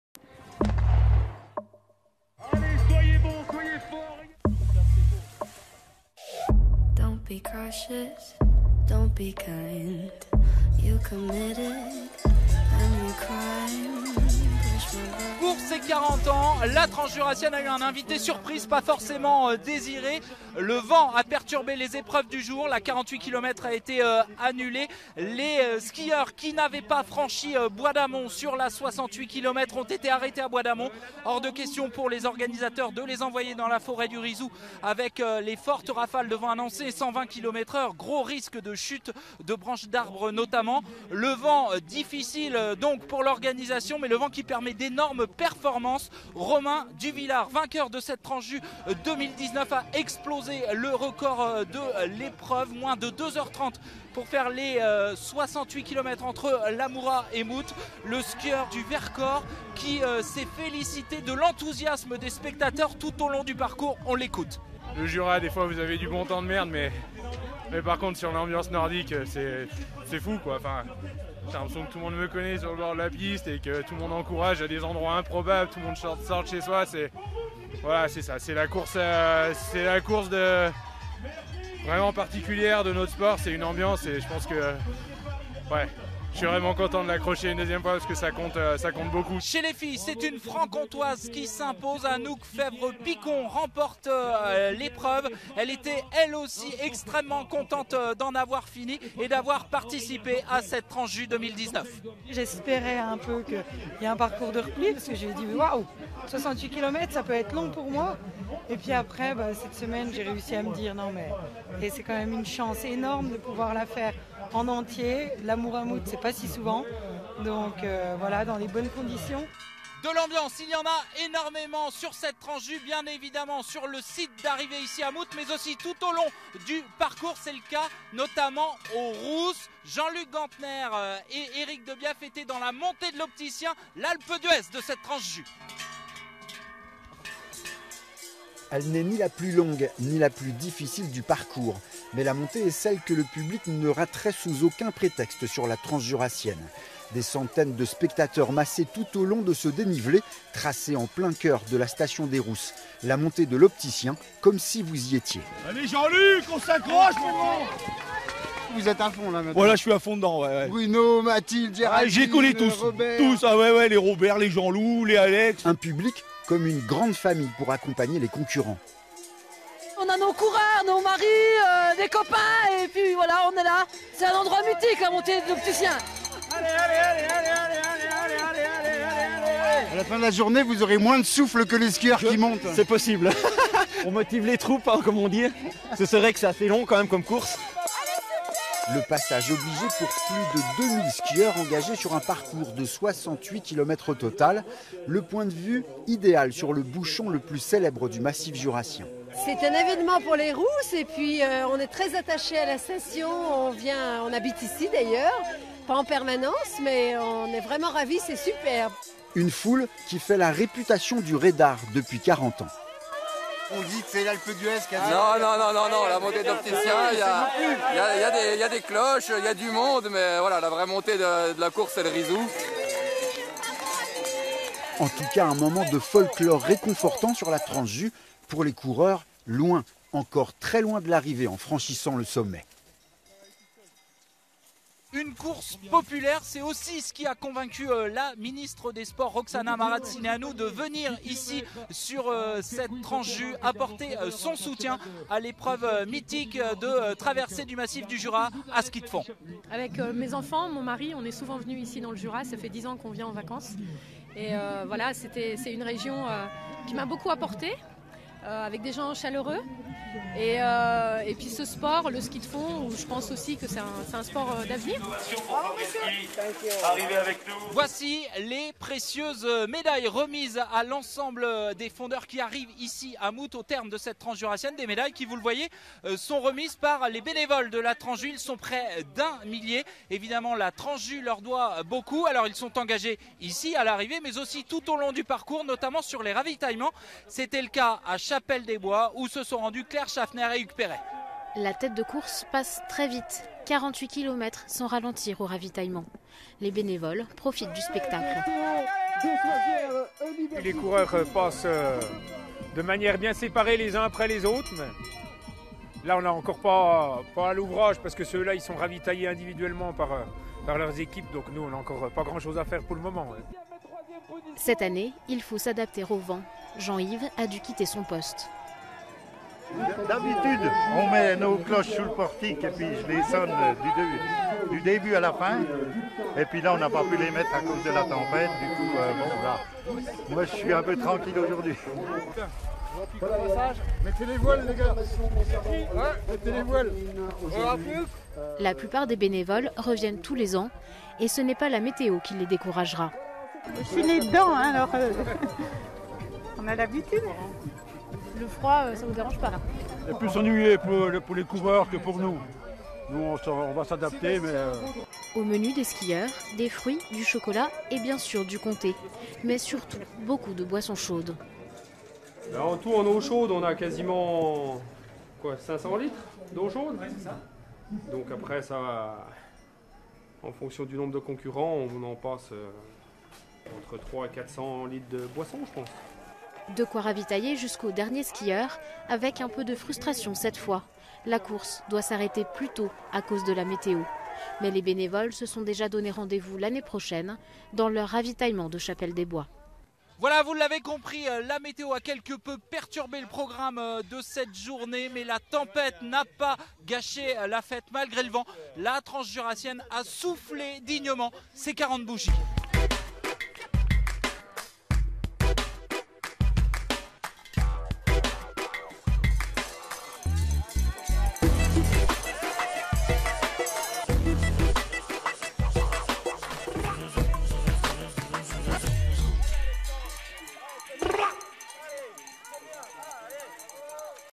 Transjurassienne 2019 : le “best of” de ce dimanche
Ce dimanche 10 février, la météo a un peu gâché la fête nordique. Une des courses annulée. Des chronos de fusée pour les vainqueurs Robin Duvillard et Anouk Faivre-Picon. Voici nos plus belles images.